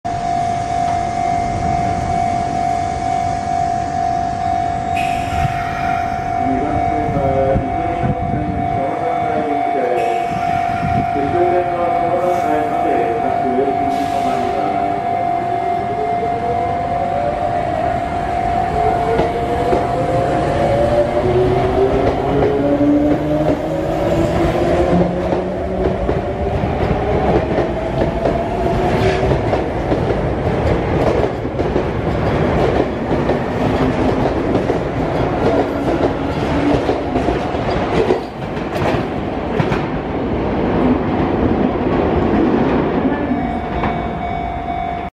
相鉄新7000系7755F 相鉄本線急行横浜行き 二俣川駅発車
今日は、忙しいので早めに投稿します。コメントを返信出来るのは、22:30以降になります。では、外出してきます。